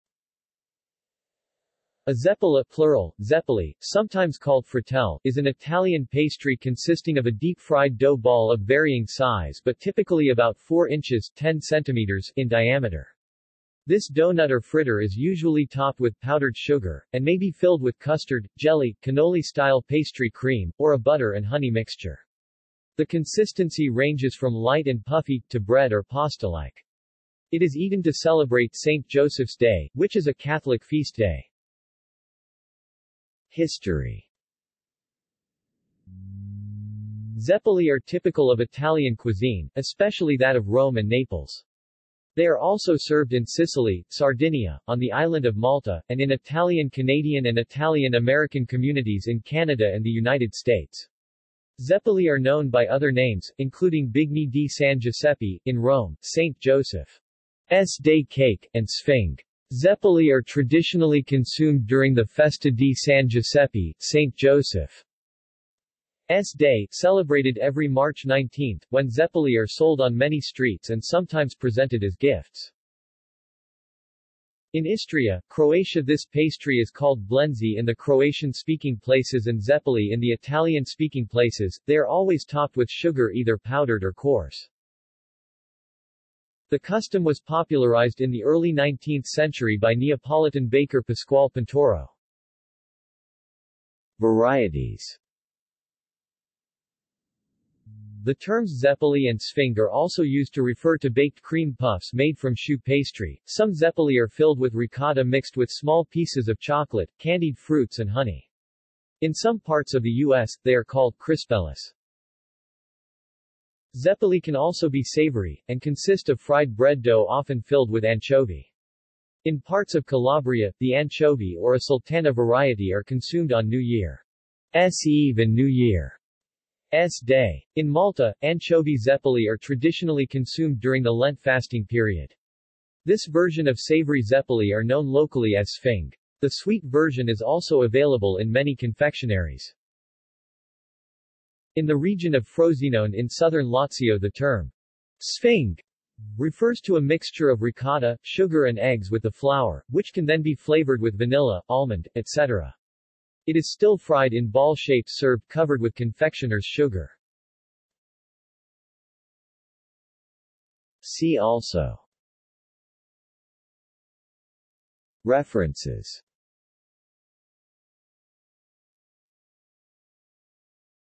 Zeppole | Wikipedia audio article
This is an audio version of the Wikipedia Article:
Zeppole

SUMMARY
A zeppola (plural: zeppole; sometimes called frittelle) is an Italian pastry consisting of a deep-fried dough ball of varying size but typically about 4 inches (10 cm) in diameter.  This doughnut or fritter is usually topped with powdered sugar, and may be filled with custard, jelly, cannoli-style pastry cream, or a butter-and-honey mixture. The consistency ranges from light and puffy, to bread- or pasta-like. It is eaten to celebrate Saint Joseph's Day, which is a Catholic feast day.